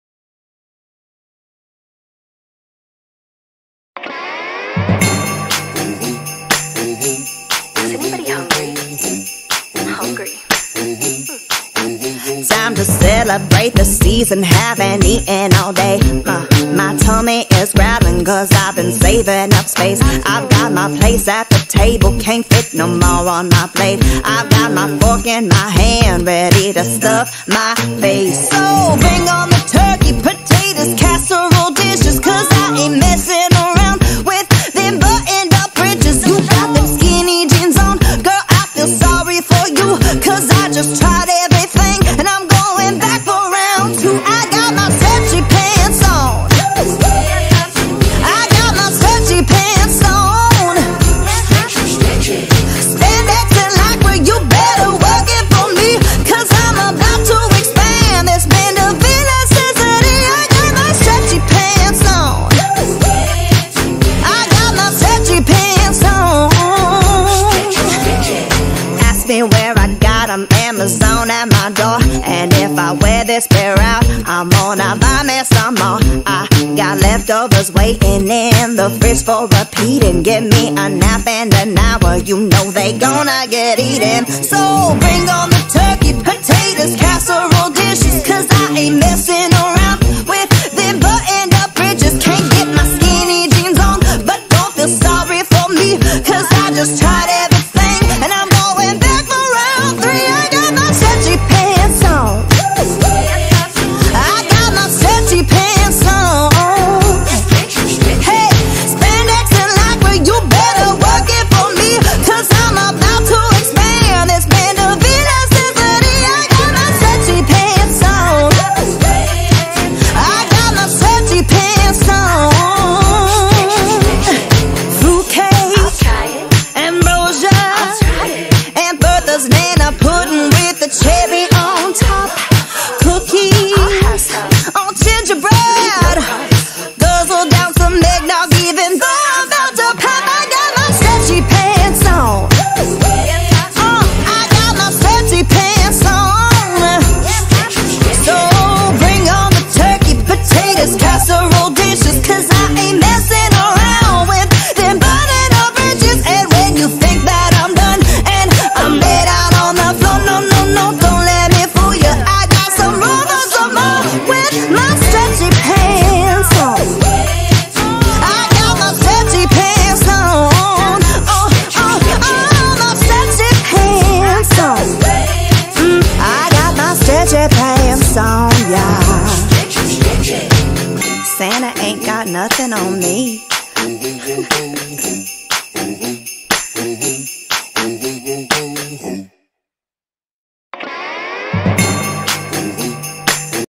Stretchy Pants Cha line dance
Choreographed by Zan Tan (SG) - Jan 2022
32 counts 2 Walls
Level : Improver Cha Cha